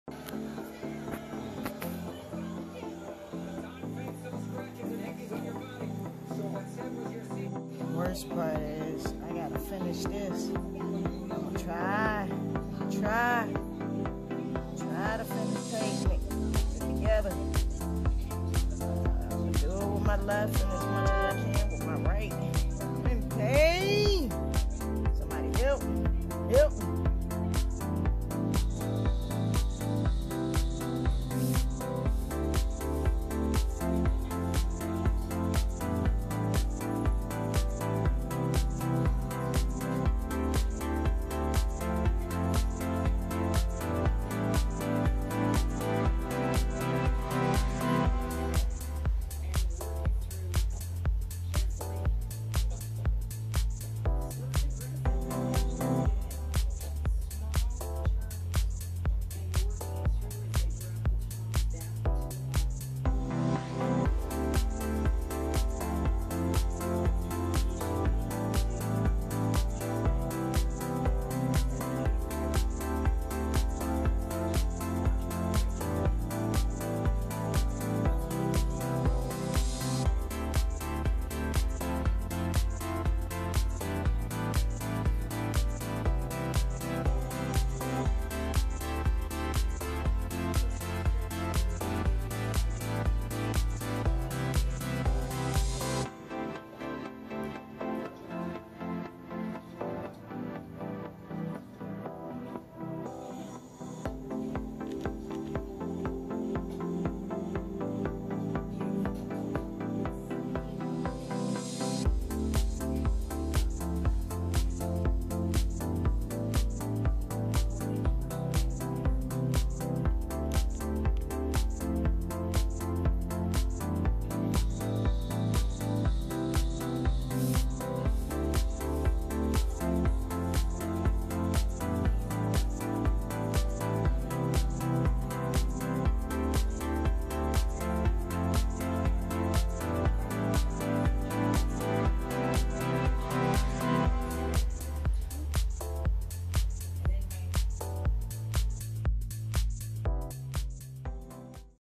3 rd surgery but business must go on
Broken my knuckle in half on my right pinky finger..I've done had 4 surgery altogether but 1 was for damage nerves...I own and operate my Solo entrepreneur. so show/work must go on..I'm right hand..wait..I'm stop at this moment give thanks for my mother, whom were left hand and taught me how to use it as a child...so no matter my right hand damage at the moment..I still used my left hand to complete a job..